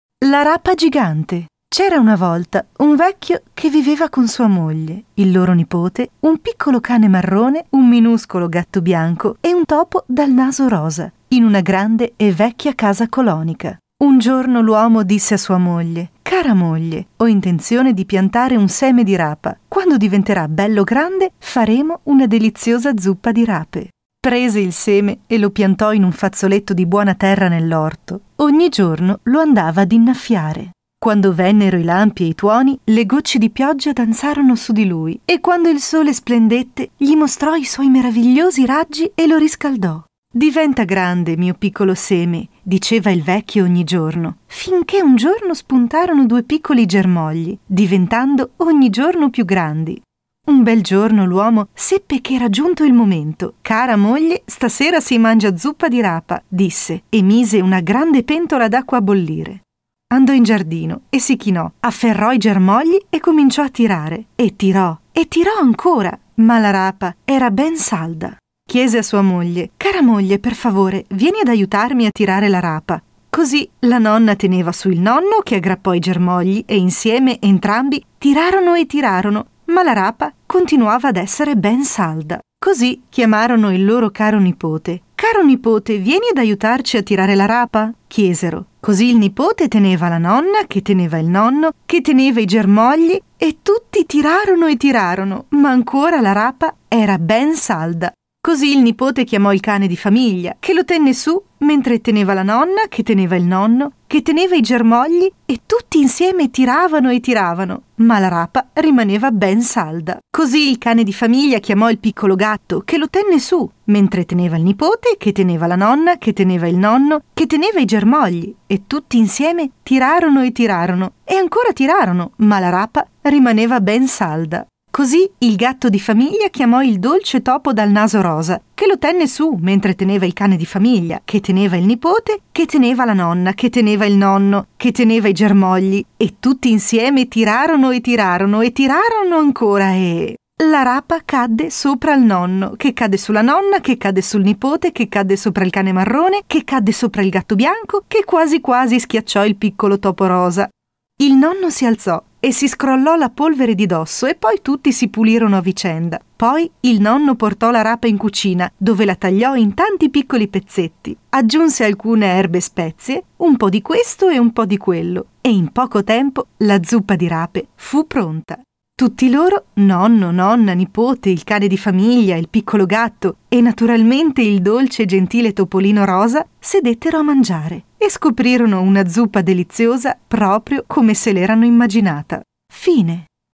La Rapa Gigante
Un anziano signore decide di piantare un seme di rapa in modo che quando crescerà potrà farne una deliziosa zuppa di rape...